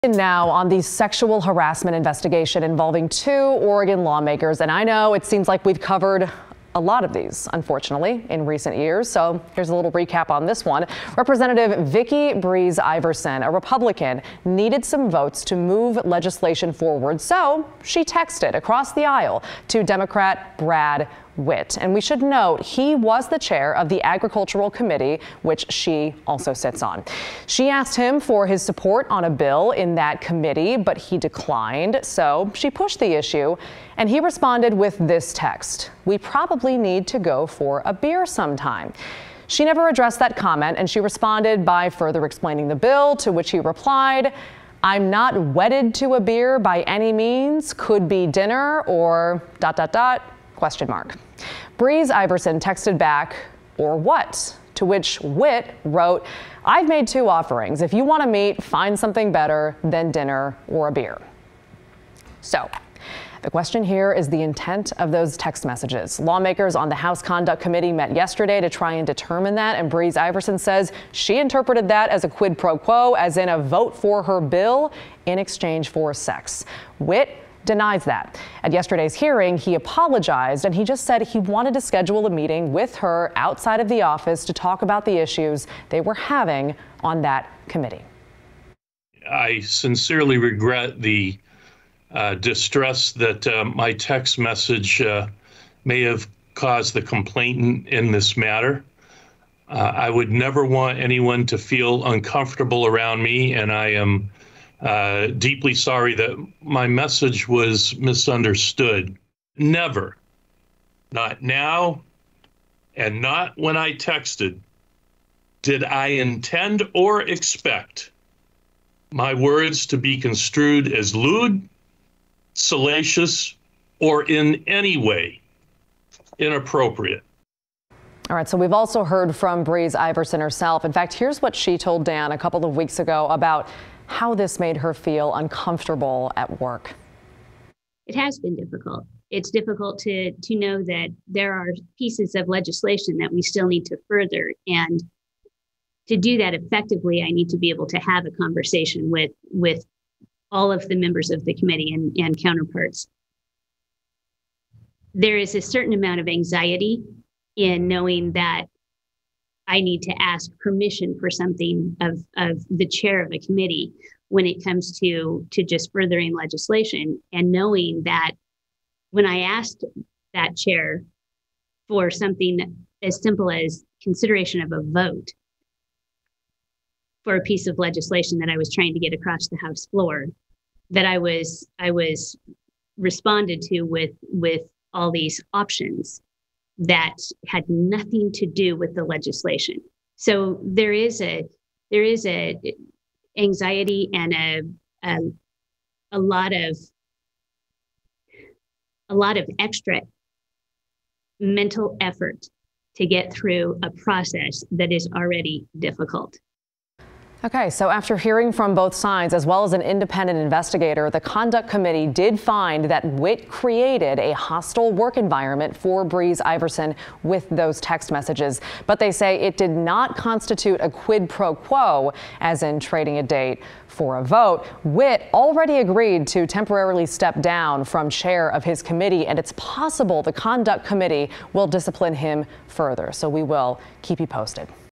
Oregon lawmaker sexual harassment investigation finds hostile work environment but no quid pro quo
Brad Witt already agreed to temporarily step down as chair of his committee and he could be disciplined further.